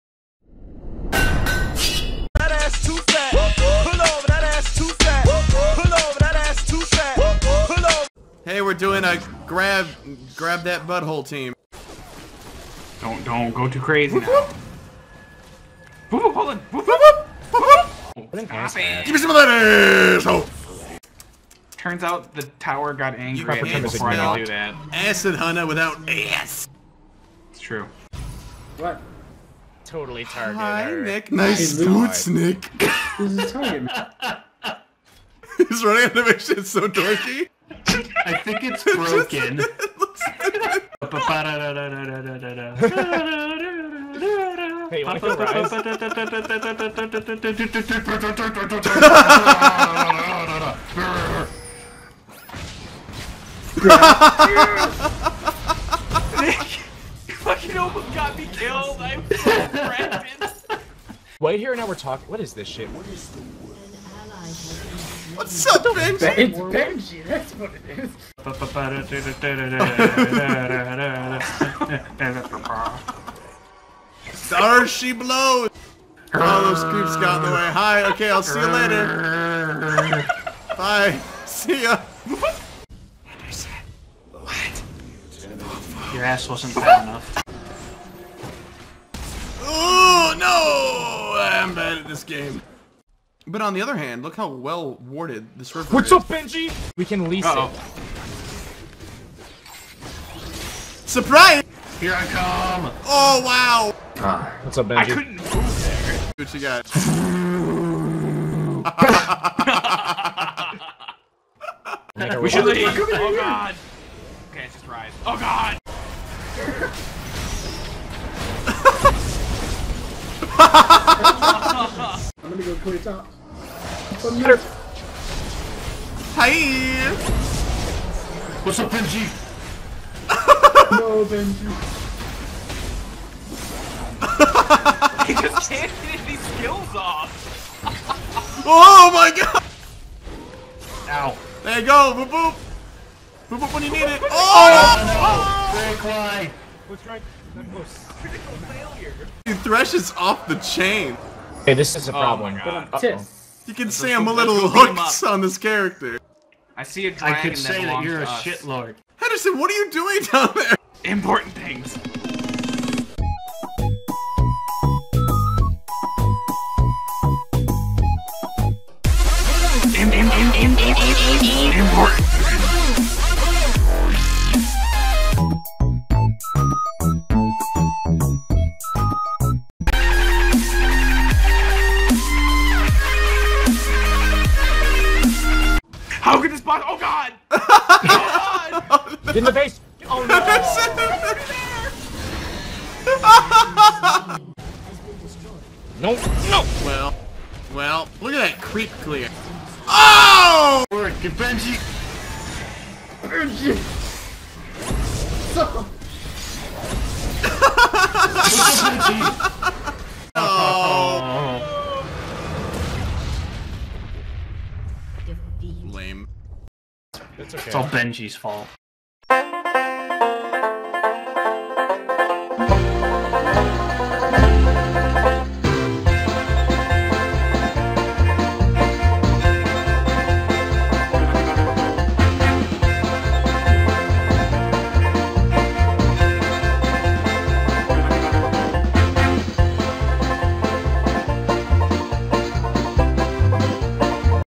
Grab Team: Gimme Sum' of Dat Butt - Leaguecast Podcast
Leaguecast's Rick, Henderson, Chris, Kevin (Posing as Whitehair), and Nick touch your toosh.